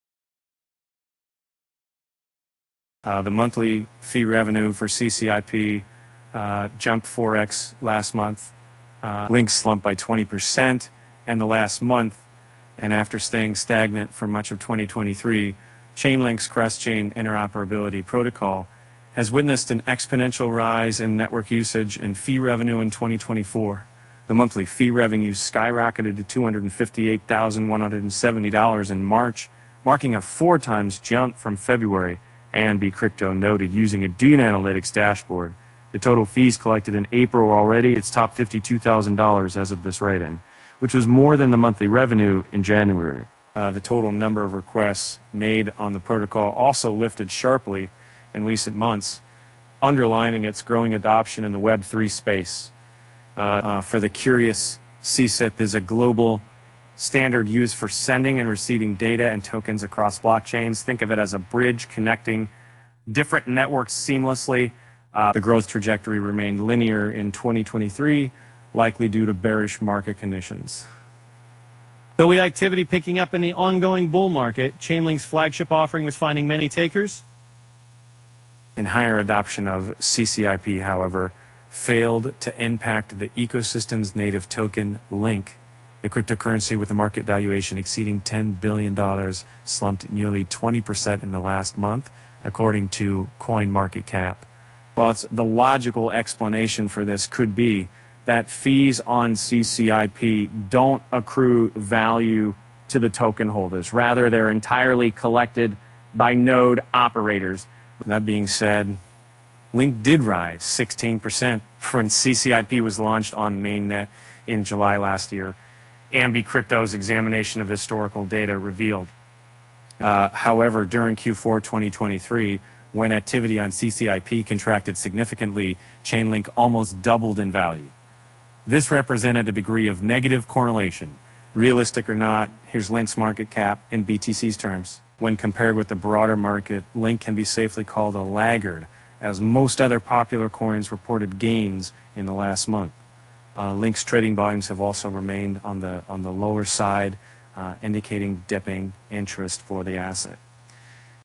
Chainlink: Checking LINK’s 20% drop amid a 4x jump in fees
After staying stagnant for much of 2023, Chainlink’s Cross-Chain Interoperability Protocol has witnessed an exponential rise in network usage and fee revenue in 2024.